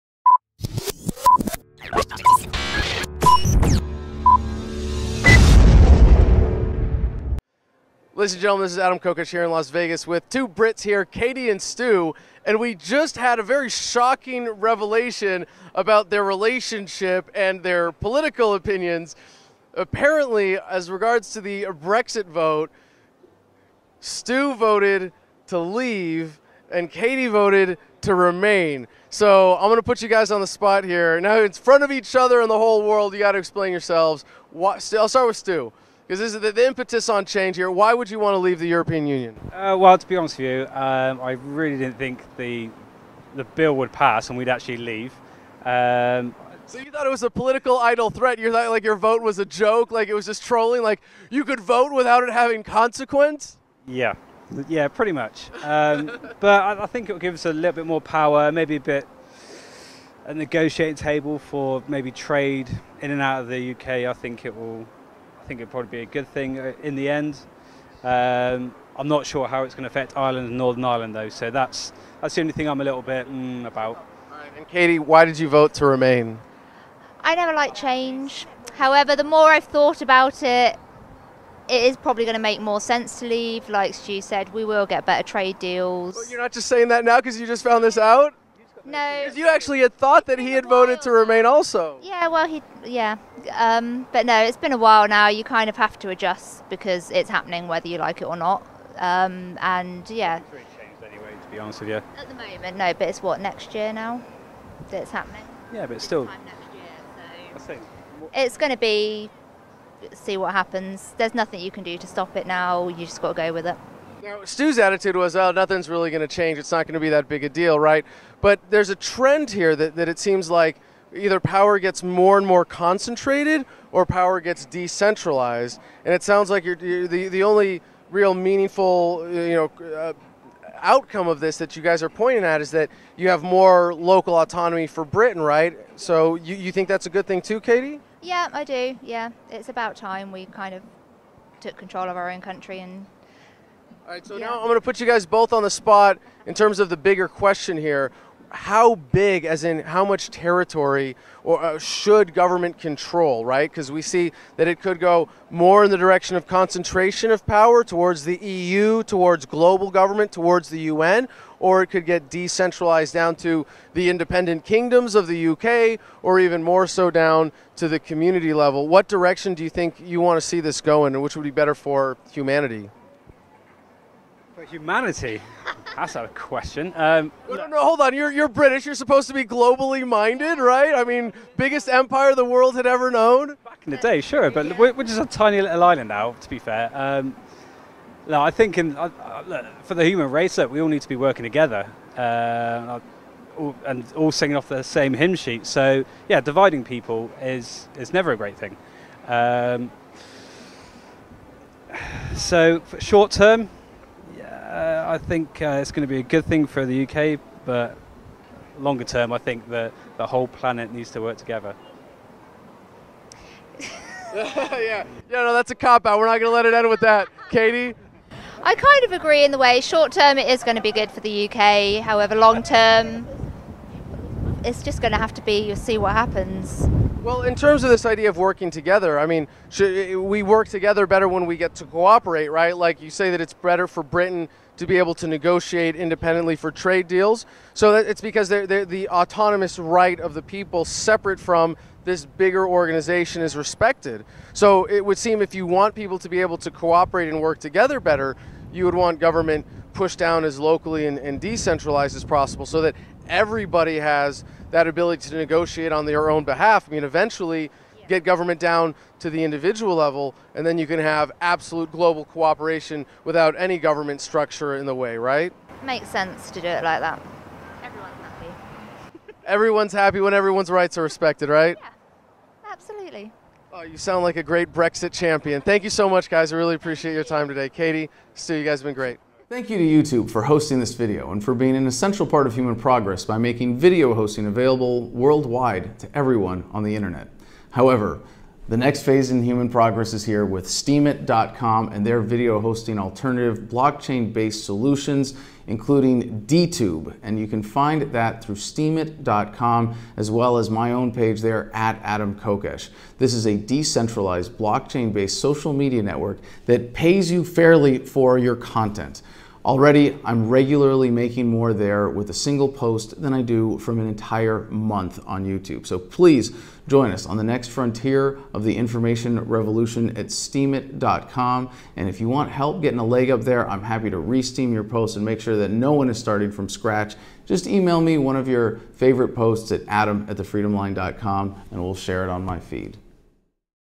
Adam finds out one of them voted for BREXIT and the other didn't!
Uh oh! What happens when this British couple finds out that they both voted for the opposite sides?